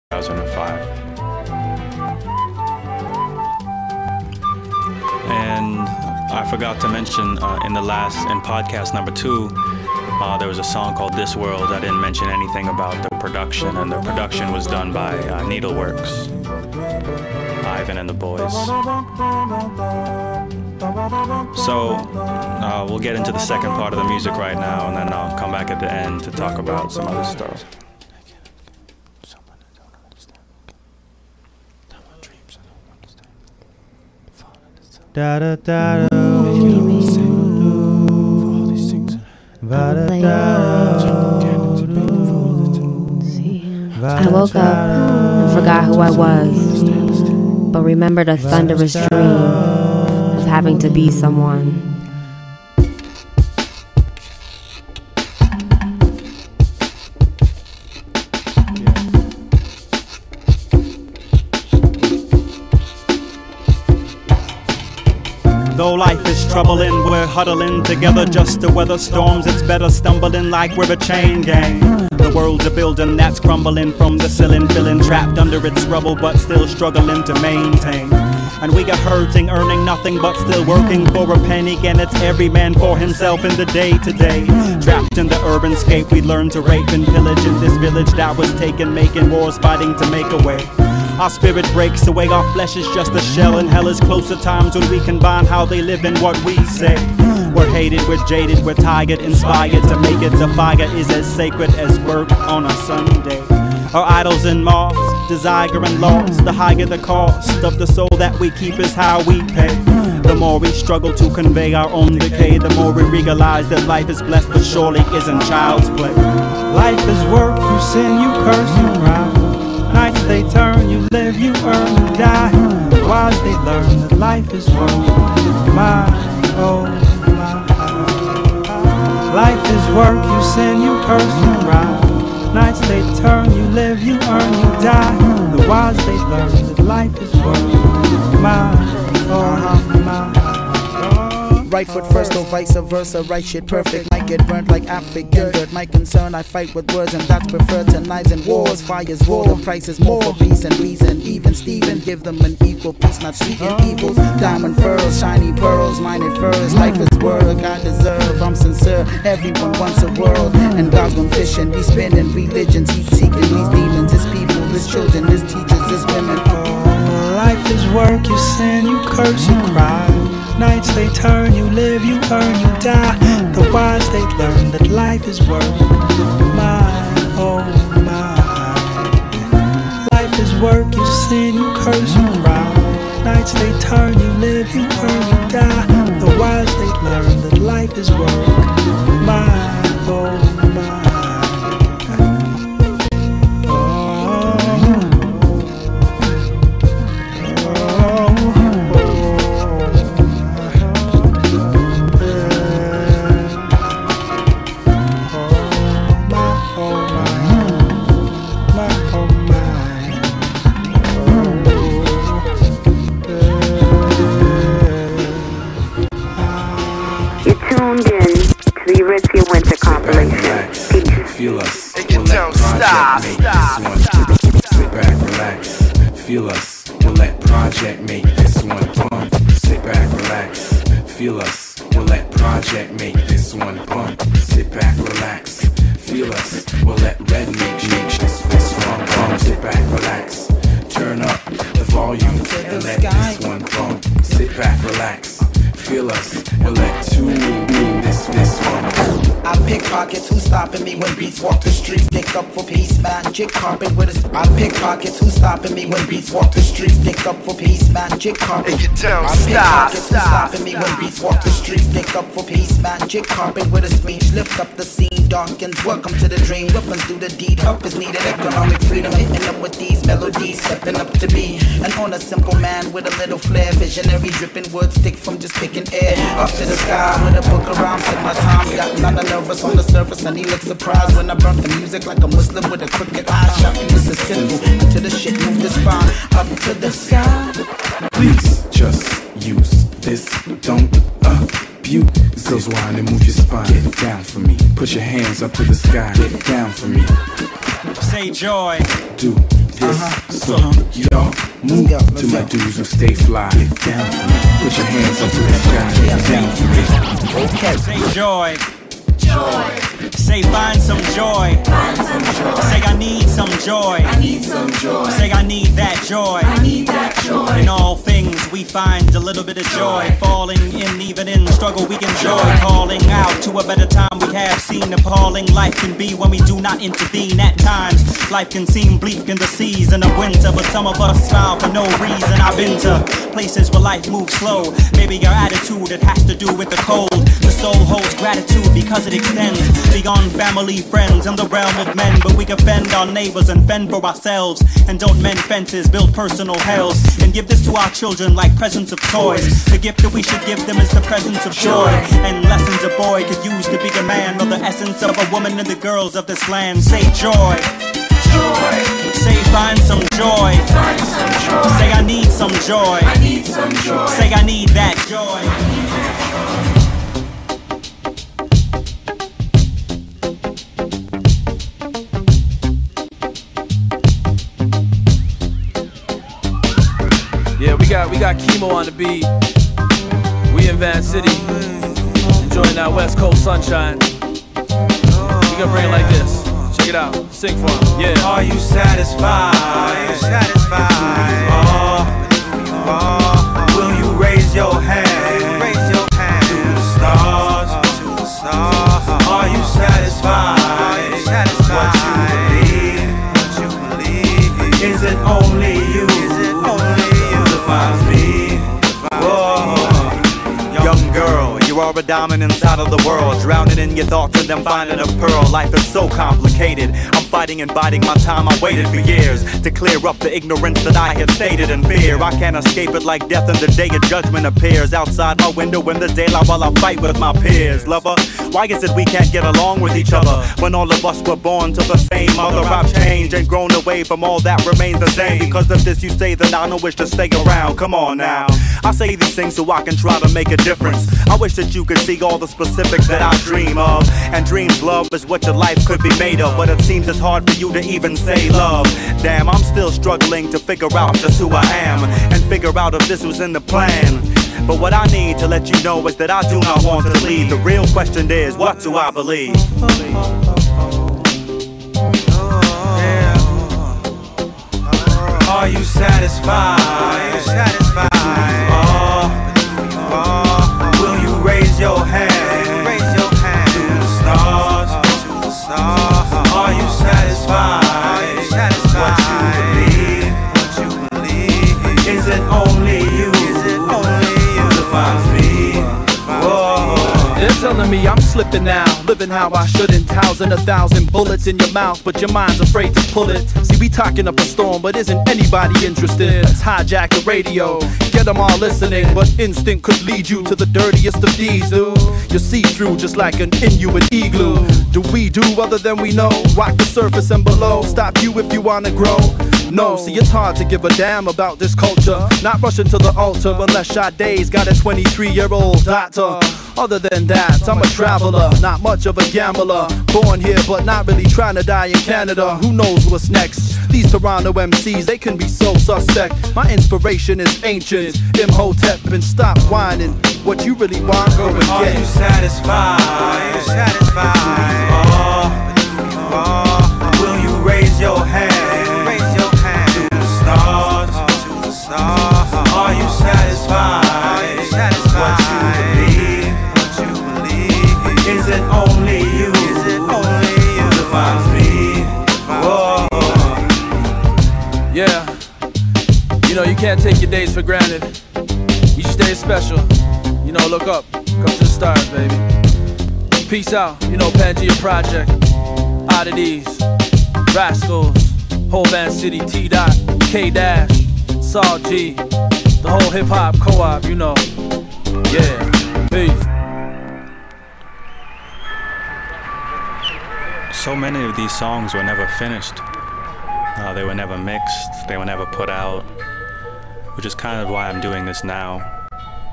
sketches3 (3 of 4)
Sketches (The Evolution of Ian Kamau) Sketches is a series of podcasts that are meant to expose people to the music, life thoughts and ideas of Ian Kamau. With original illustrations by Jabari Elliot (Elicser) the five part series was originally released on a monthly basis from January to May 2009 in preparation for the release of the September 2009 album release.

.album september 2009 ...mixtape june 20 '09 ... iankamau.com coming soon